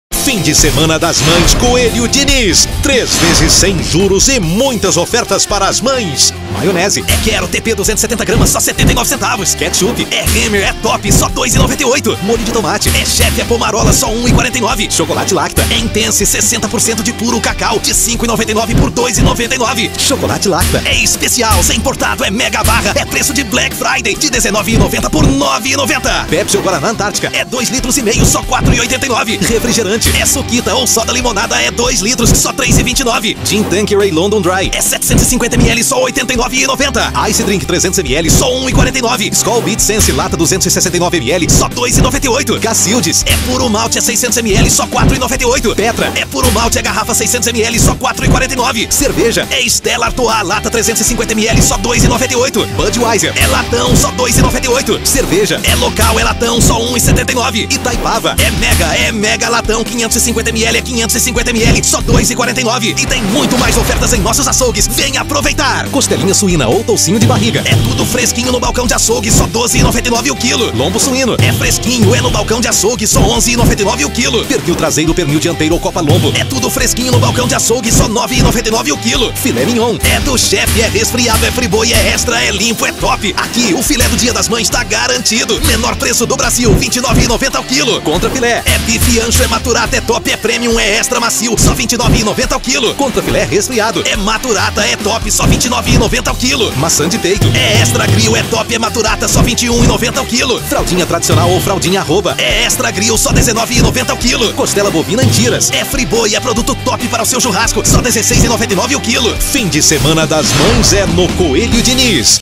Coelho Diniz - ofertas válidas de 08 a 10 de Maio de 2020
Fim de semana das mães é no Coelho Diniz porque afinal, elas merecem o menor preço! Venha aproveitar as melhores ofertas do Leste de Minas e conferir o festival da carne em até 3x sem juros!

Ofertas válidas para sexta, sábado e domingo 08 a 10/05/2020